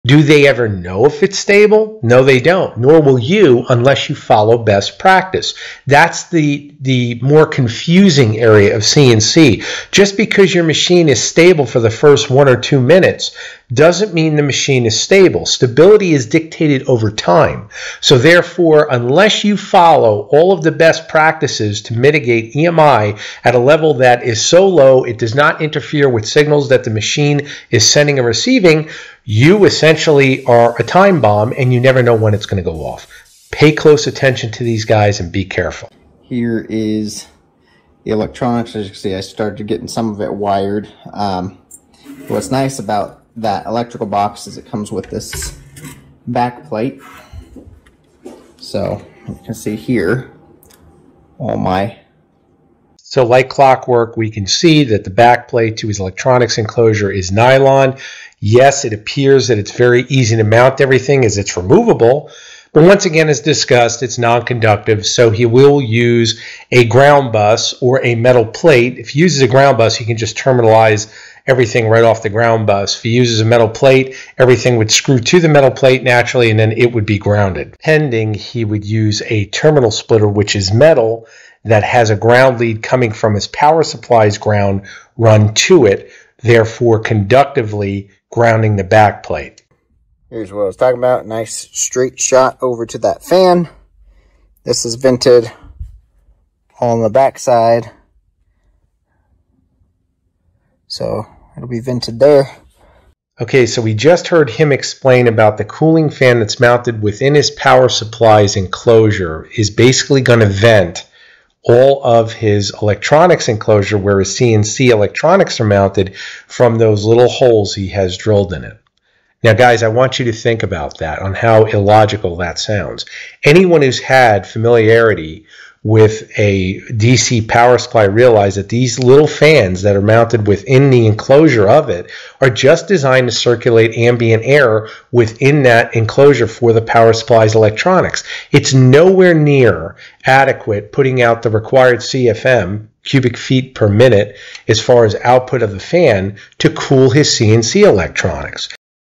🔥 CNC Electronics DISASTER! No Grounding + No Cooling = FAIL (Engineer Explains)
As a professional robotics engineer, I’m exposing critical flaws in this CNC electronics build that could lead to catastrophic failure! The creator is using a non-conductive controller (no grounding!) and has zero cooling fans—a recipe for disaster. In this video, I break down:

⚠ Why EMI shielding & grounding are NON-NEGOTIABLE for CNC stability
🔥 How overheating destroys electronics (and why his fanless setup is dangerous)
🔧 Proven cooling solutions to keep your CNC running smoothly for years
🚩 How to spot bad CNC advice from amateur creators

Don’t risk fried circuits or failed cuts—learn how to build a reliable, high-performance CNC the right way!